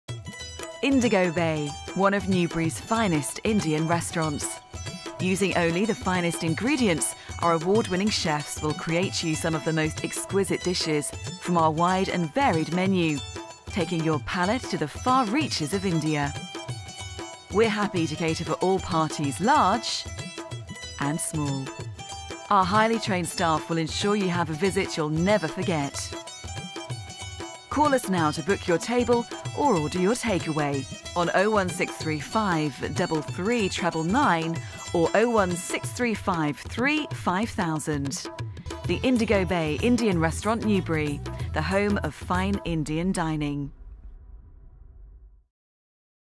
INDIGO BAY - NEWBURY
about indigo bay

Indigo Bay has recently opened its 3rd restaurant in Berkshire . Jamal & Khanus Miah, have concentrated the experiences of their 30 years in the industry into creating a substantial menu of self-styled Indian and Bangladeshi dishes, as genuine as any served this side of the Orient.

Whether you like your food hot, mild or Westernised there's plenty to choose from, such as shamdo chicken (medium hot) and ramo chicken (hot), cooked in an iron souk - both recommended by the chef. Children might prefer the very mild malai chicken tikka, created with their younger palates in mind. Painted mainly in cream colour with original Indian furniture decorated with flowers, there's a modern feel to the place.